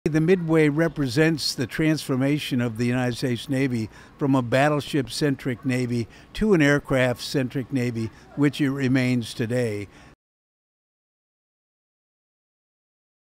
Is the US Navy aircraft or battleship centric? l USS Midway Museum CEO Mac McLaughlin explains
The US Navy of today is aircraft-centric…but nothing happens without a ship. And for a carrier group that means a bunch of ships.

Fran Racioppi traveled to San Diego Harbor to take over the Hanger Deck of the USS Midway Museum for a conversation with the founding CEO RADM (ret) John “Mac” McLaughlin on the the importance of today’s Navy, the role of the aircraft carrier and how he built a museum out of one of America's biggest ships.